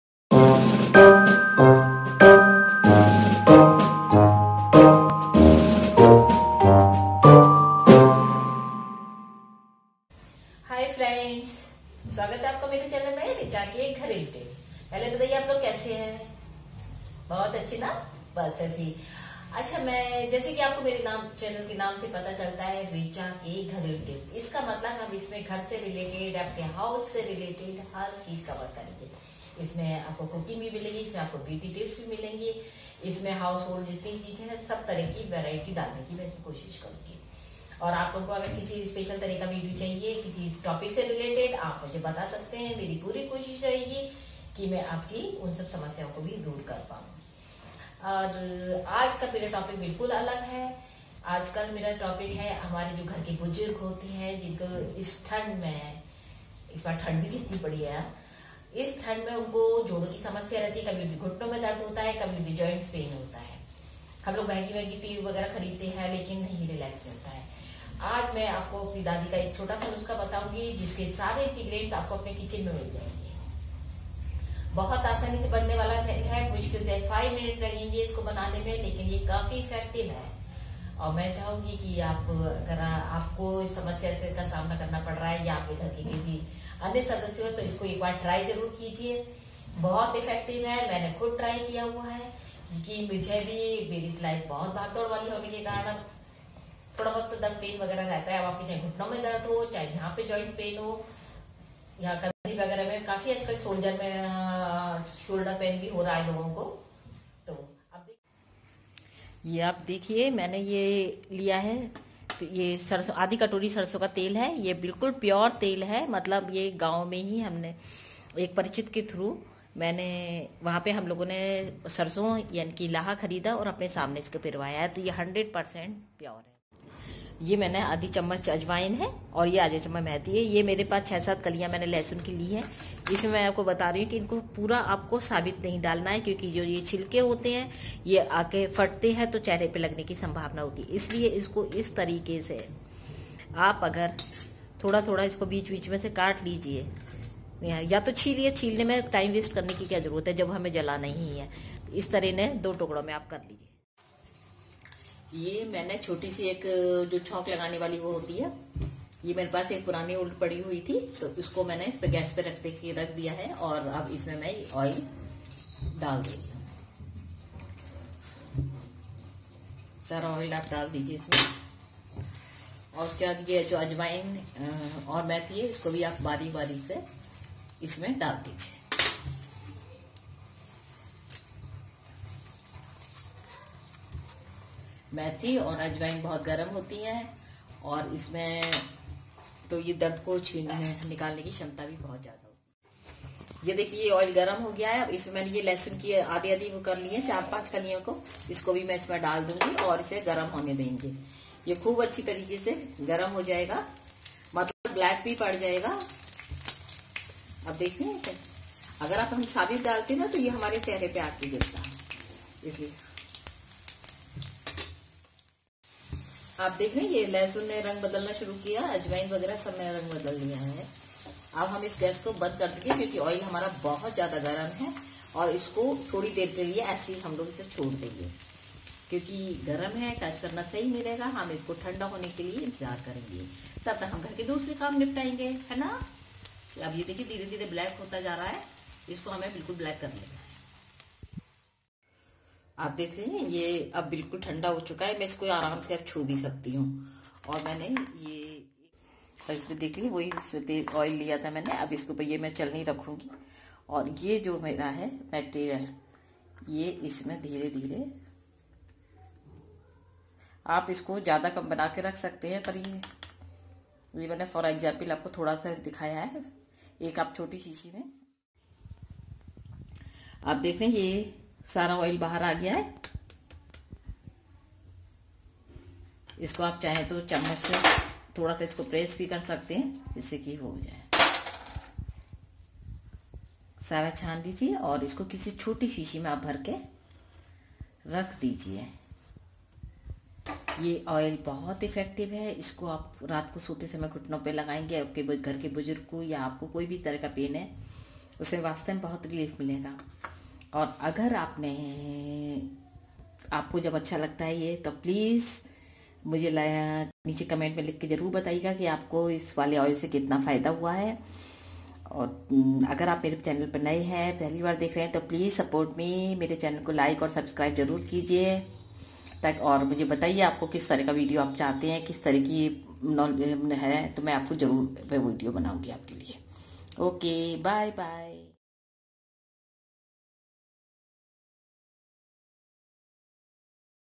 Sardi ho ya jodo ka dard... Ho jayega furr
Very effective oil... Sardi hone pr raat ko sote time
Baccho k seene pr malish kre.. Jodo k dard m bhi bahut aaram milega...